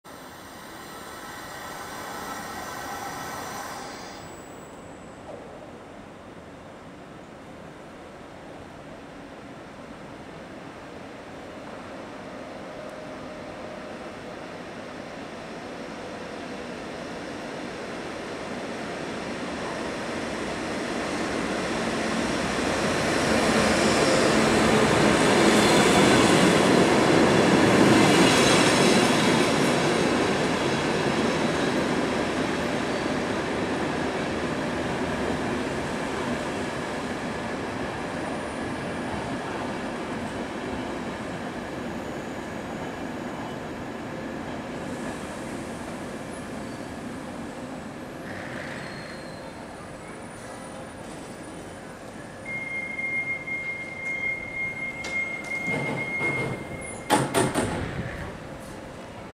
EuroCity 140 HORTOBAGY Wien Hbf
EC 140 Zahony - Wien Hbf. První vůz Kiev Pass - Wien Hbf. Zpoždění 5 min. Video pořízeno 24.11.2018.